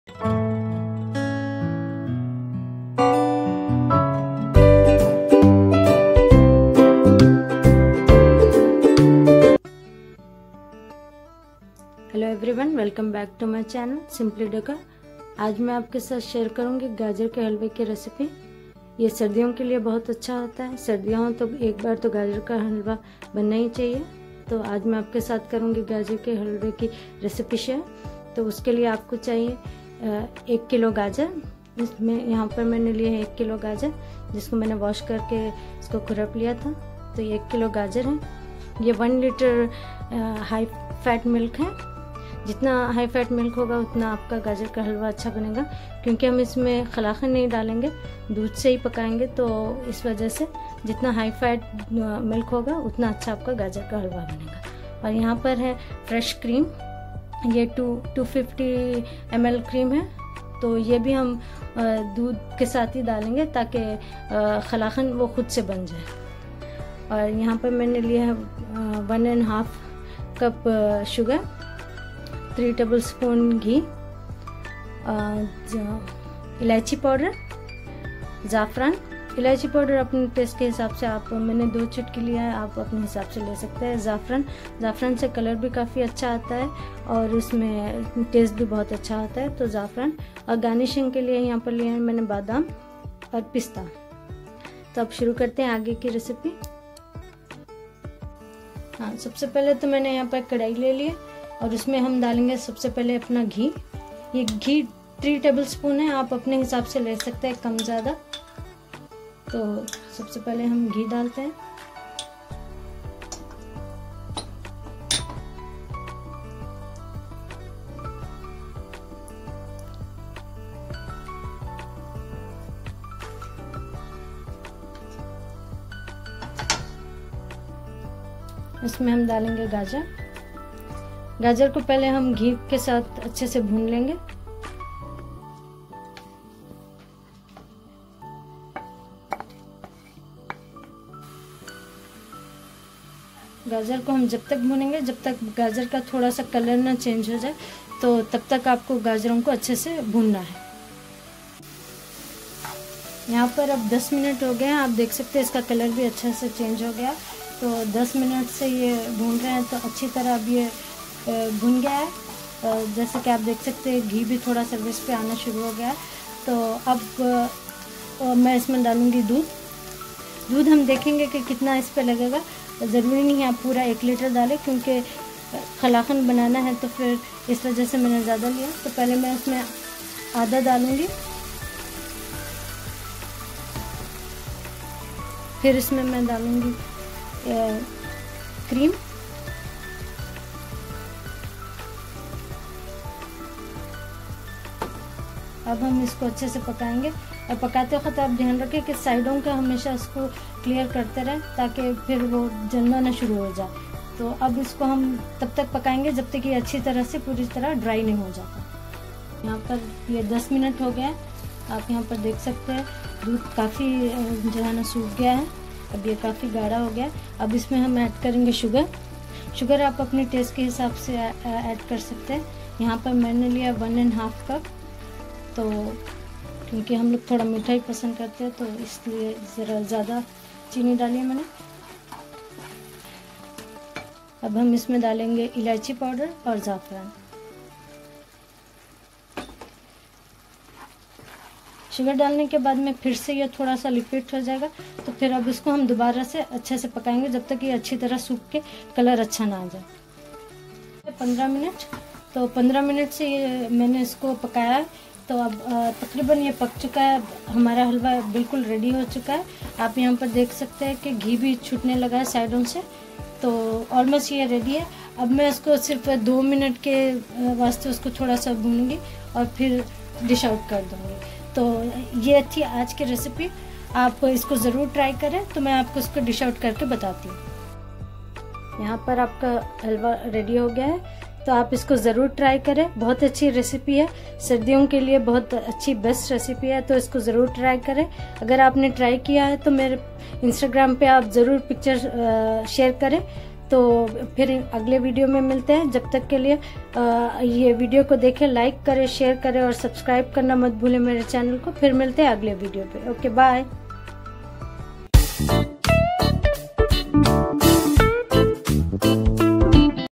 How to make gajar ka halwa | Winter RecipeBook | ep2 | Simply Decor
In this video you will learn how to make gajar ka halwa this is a really good dessert/sweet for the winter because you eat it warm.

Ingredients:

- 1 kg carrots
- 1/2 L high fat milk
- 250 ml fresh cream
- 1 1/2 cup sugar
- 2 tbsp ghee
- a pinch of cardamom powder
- safron
- almond
- pichtachio

Simply Decor...